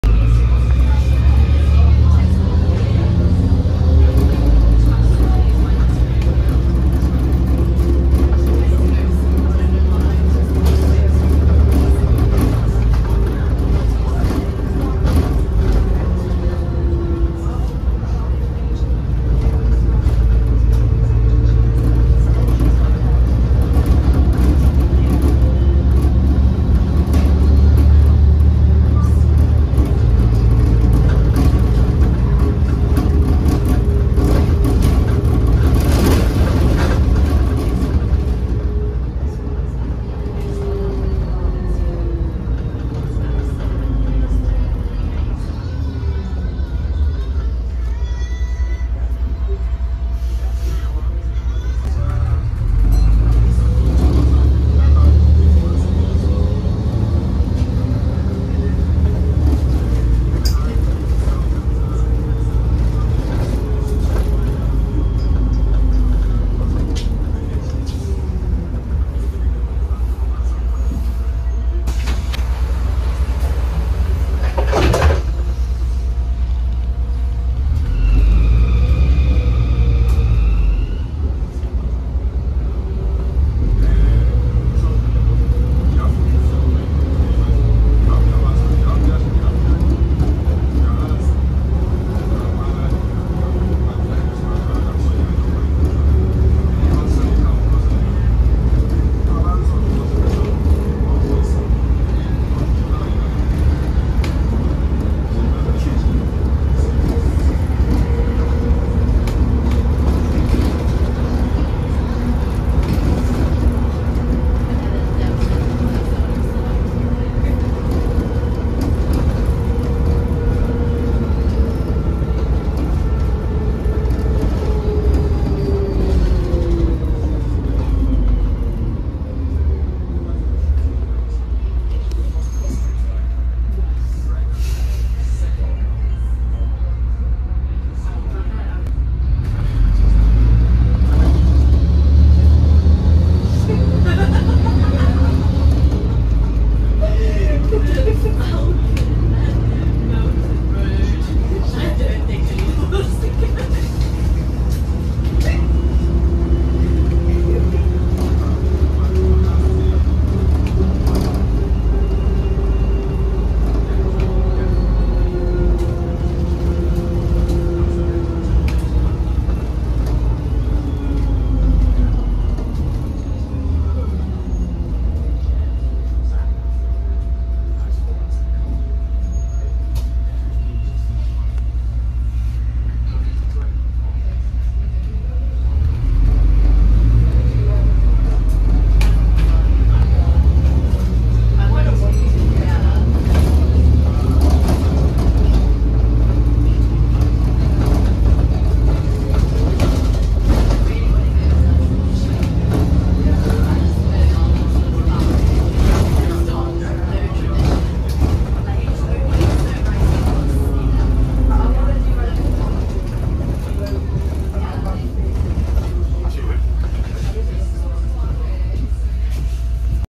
*SCREECHING FANBELT* Ride on Arriva VDL/DAF DB250 Wright Pulsar Gemini 4777 (YJ56 KFL) Route 44
This is my fifth bus ride on the Saturday I went out.

Here is a short ride of Arriva Midlands 4777 serving the route 44 to City Centre. 4777 has started to develop a screeching fanbelt when the handbrake gets released and pulling away.

Filmed on Saturday 17 February 2024

Filmed between Victoria Park to Leicester